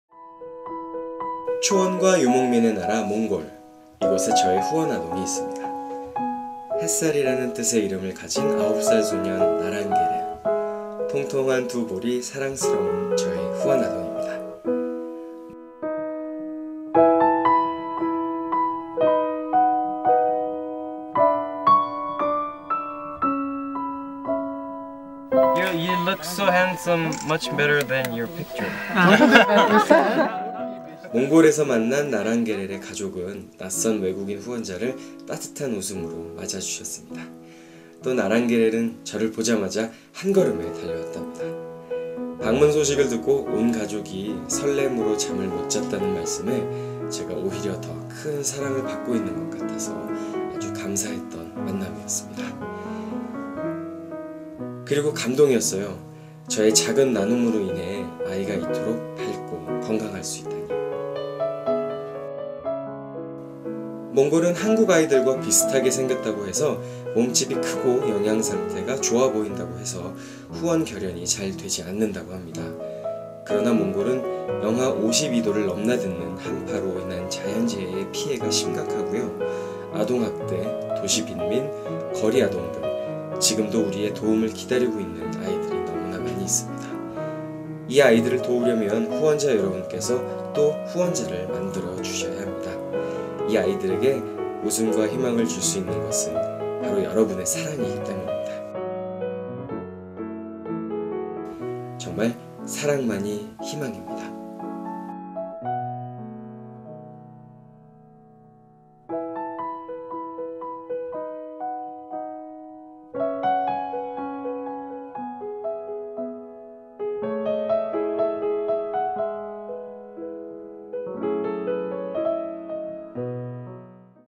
[월드비전] [사랑만이 희망입니다] 몽골 이기찬 후원자
언덕 꼭대기에 있는 나랑게렐 가족의 게르에도 물 양도이 두 개가 놓여 있었다.
식수로 쓴다고 했다.
누렇게 변해 있는 천막 지붕 위는 천뭉치와 나무판자가 덧대져 있었다.
한눈에 나랑게렐을 알아본 이기찬 후원자가 '센 베노'라고
몽골말로 인사를 건네자
아이는 세상에서 가장 큰 웃음을 지어보였다.
이기찬 후원자는 아이를 번쩍 들어 올려 품에 안았다.

'사랑만이 희망입니다'_#7. 이기찬의 몽골